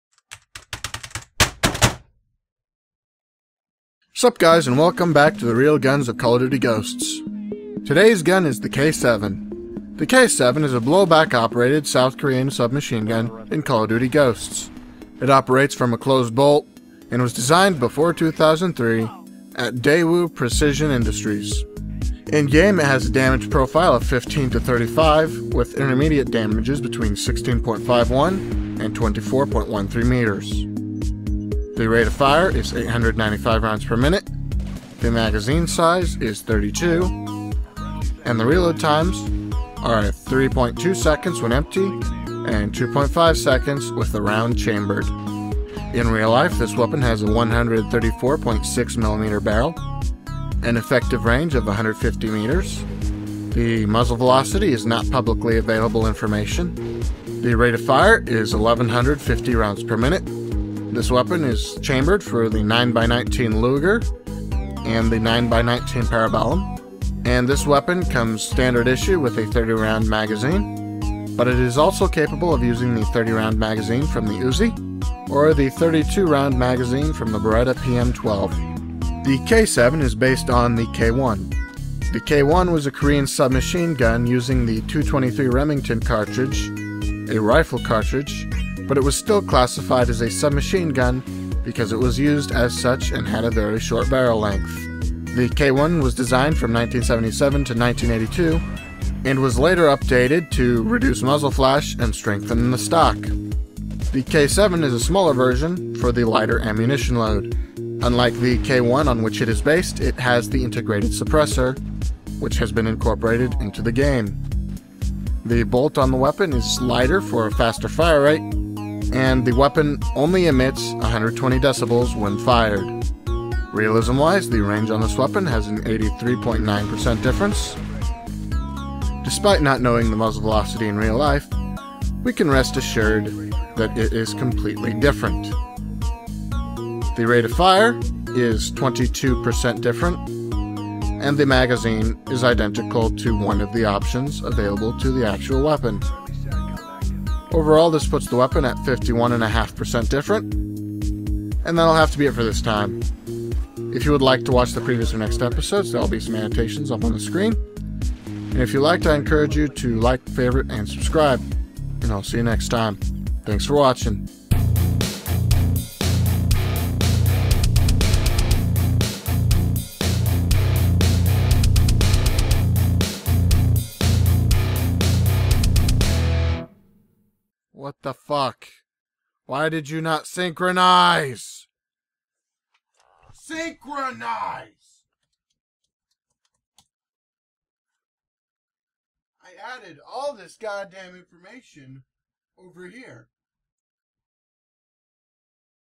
The Real Guns of COD10 — K7
The K7 is a Korean submachine gun designed for special purpose use.  It is based on the K1 submachine gun, which despite firing a .223 rifle cartridge, was classified as a submachine gun by virtue of its short barrel and usage as an SMG.